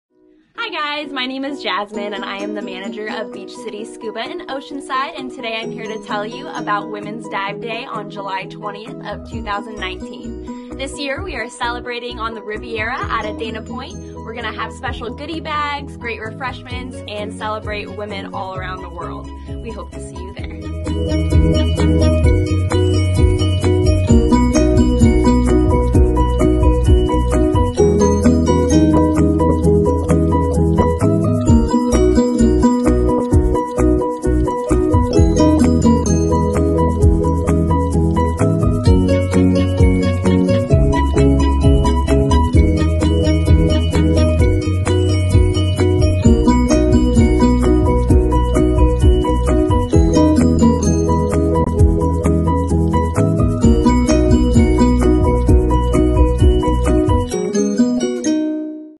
Jazmin’s Dive Locker!
Enjoy this special edition of D.J.’s Dive Locker as our manager Jazmin talks about the 2019 Women’s Dive Day!